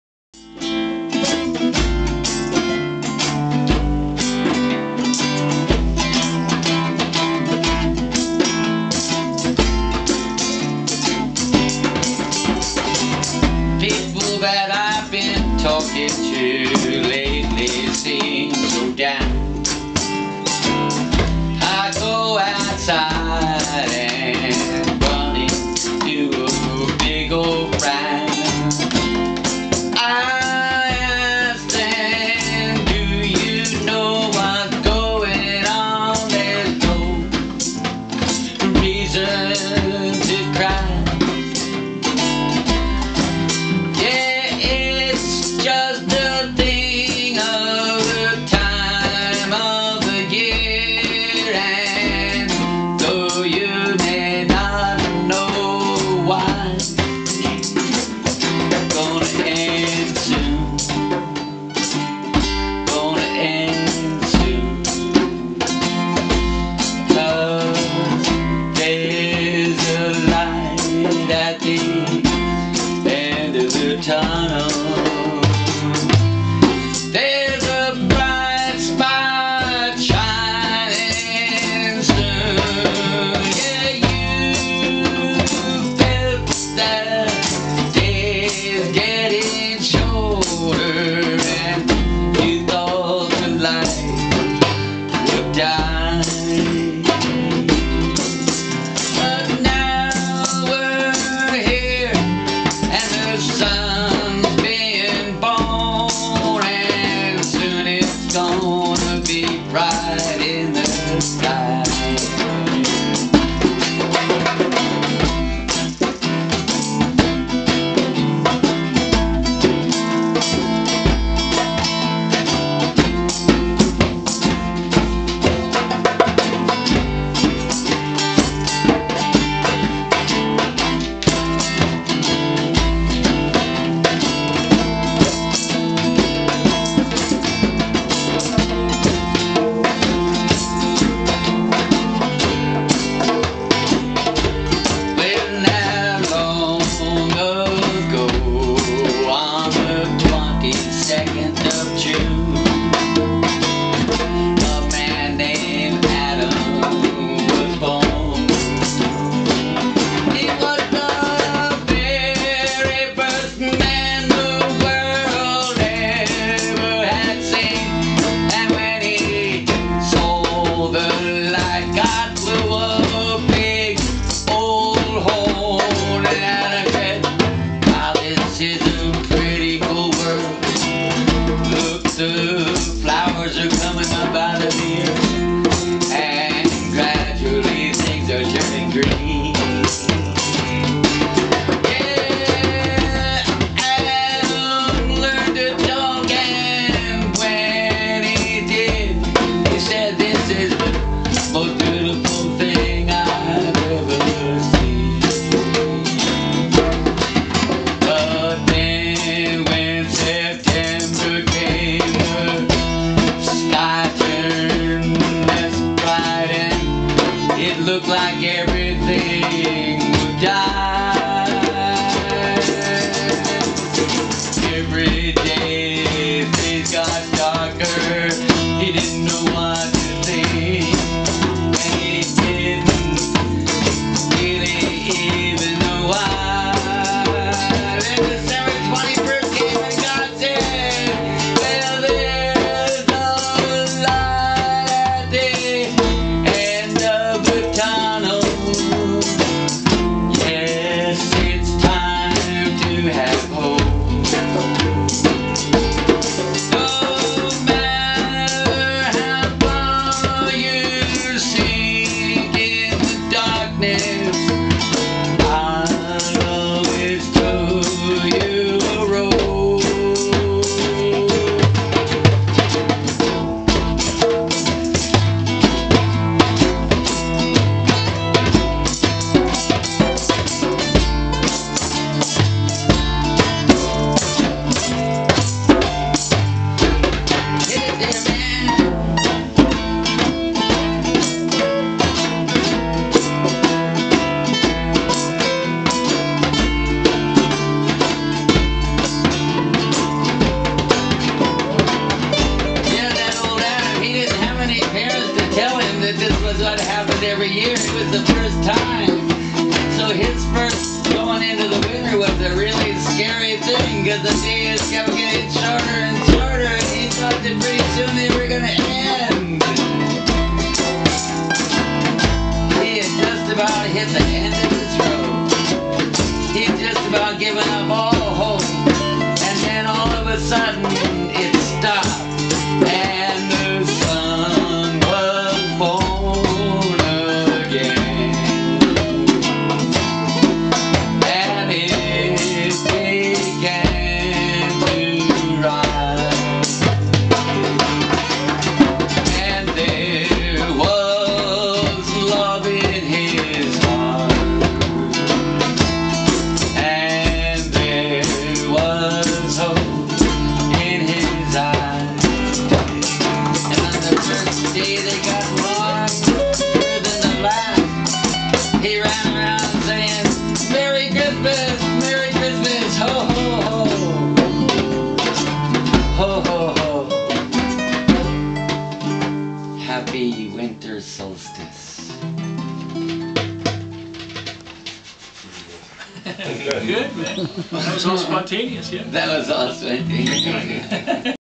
Spontaneous Happy Winter Solstice song 2015
Just winging it. What was it like for the first person on Earth with human consciousness to experience the shortening of days and then the solstice? Glenn, Andy, and Richard playing along.